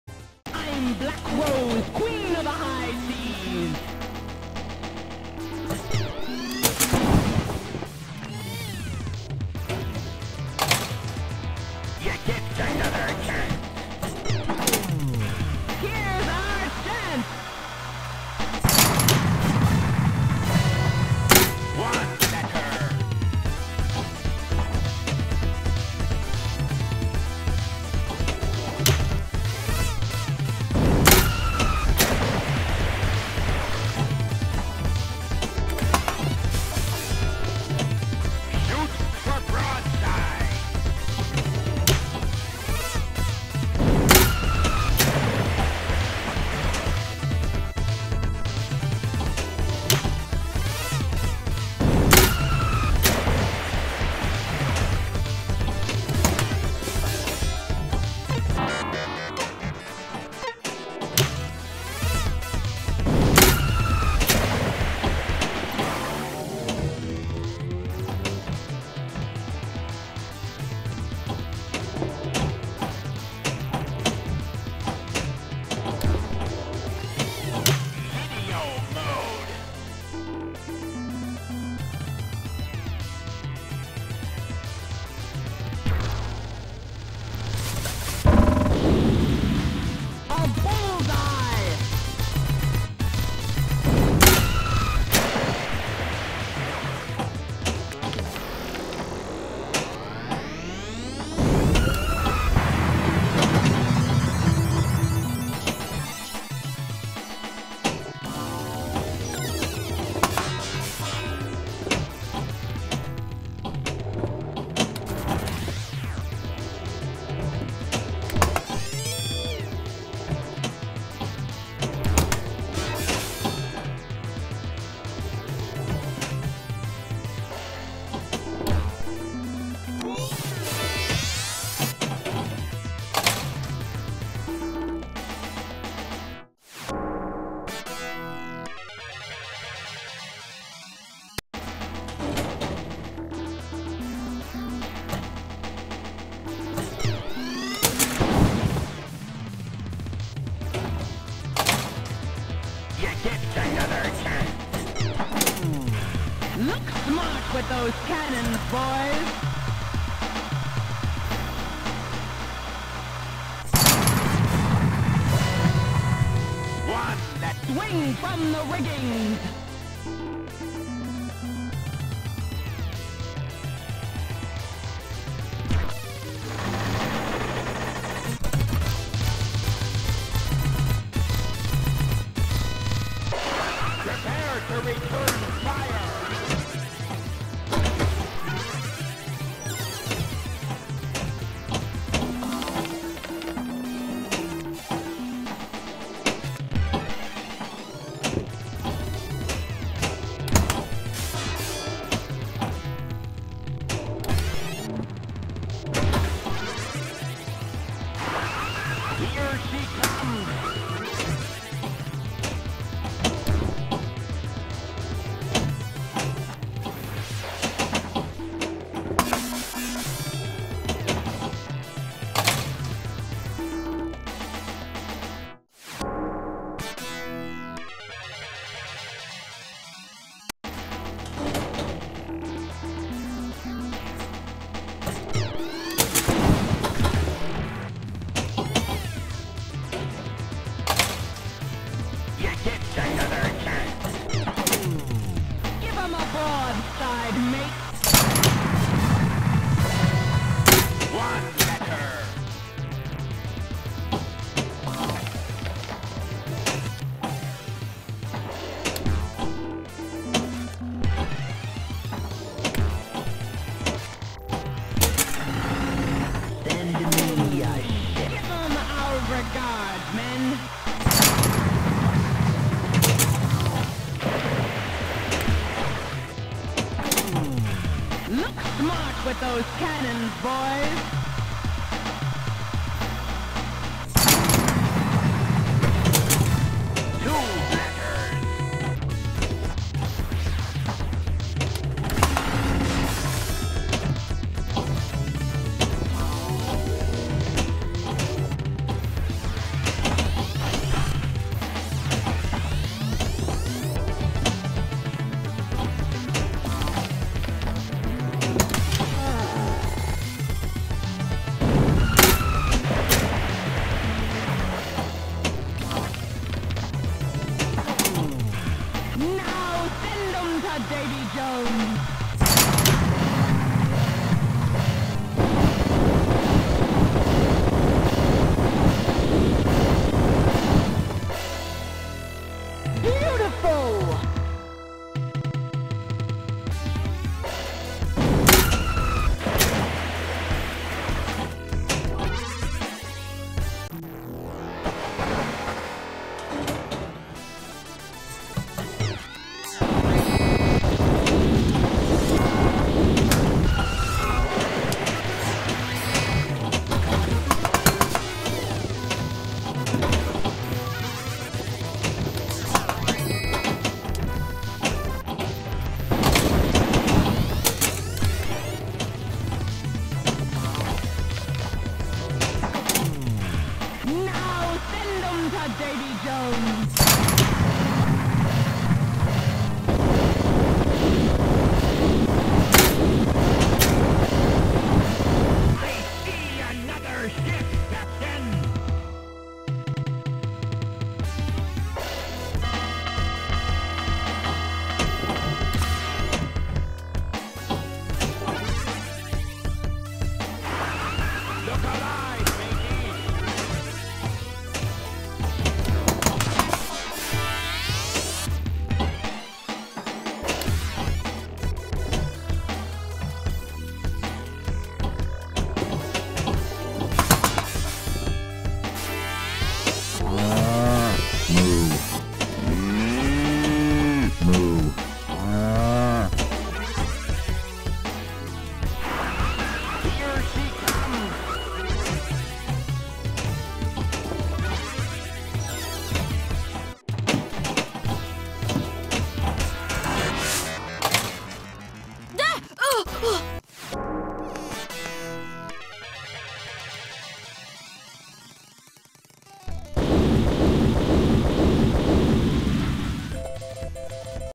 A decent game on Black Rose
I only saved this replay because my score glitched for league play, it was a good game for me, but not my best.
I edited out the score glitch.